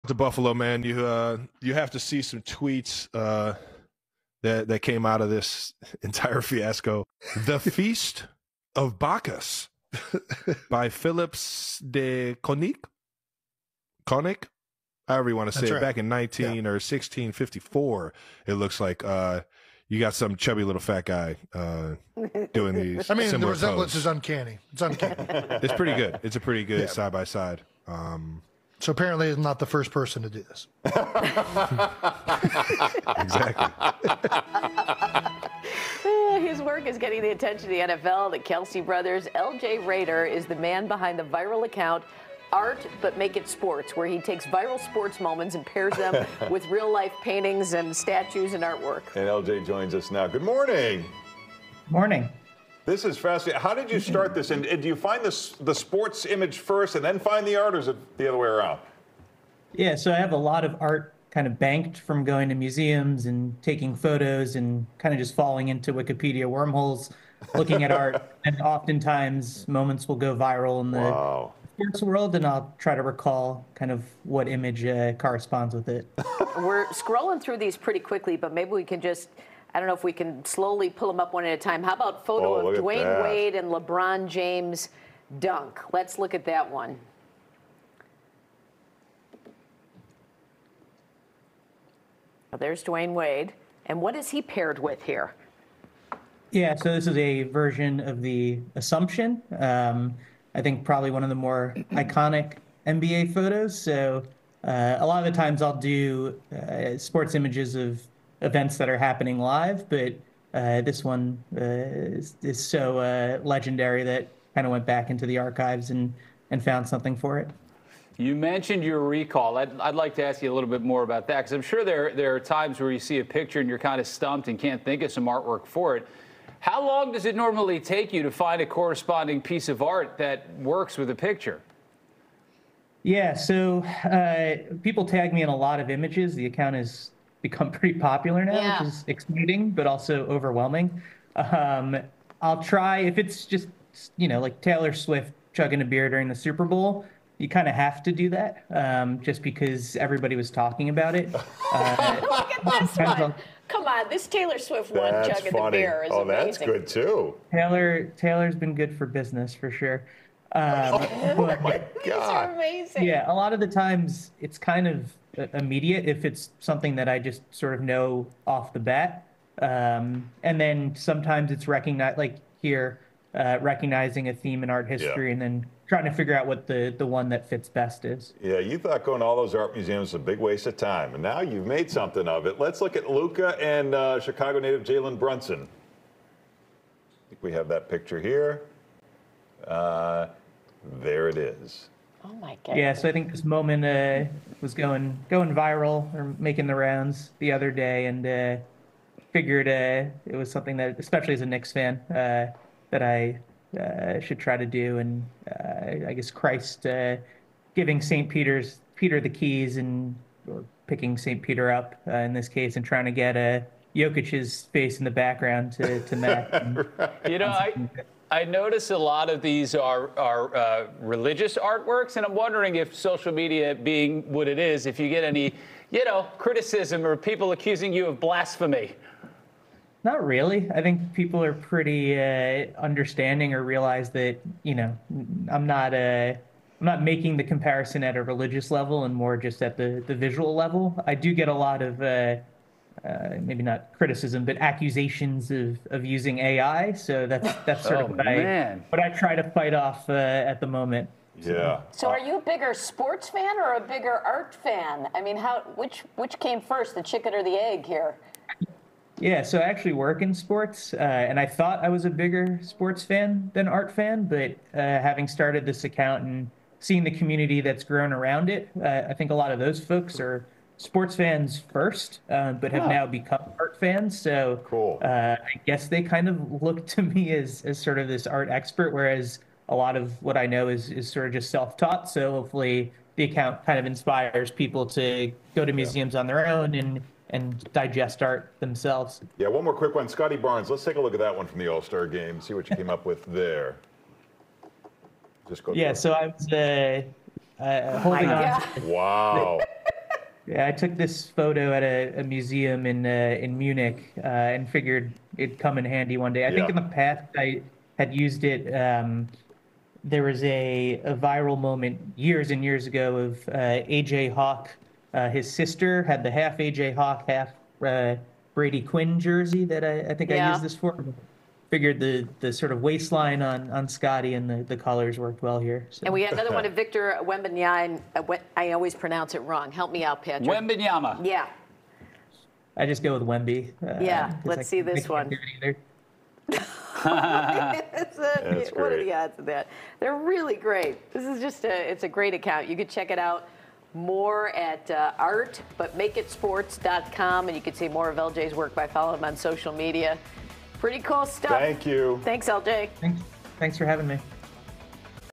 'Art But Make It Sports'
LJ Rader is the guy behind the viral "Art But Make It Sports" social media accounts ... where he pairs photographs from the world of sports with paintings and other pieces of art that mirror them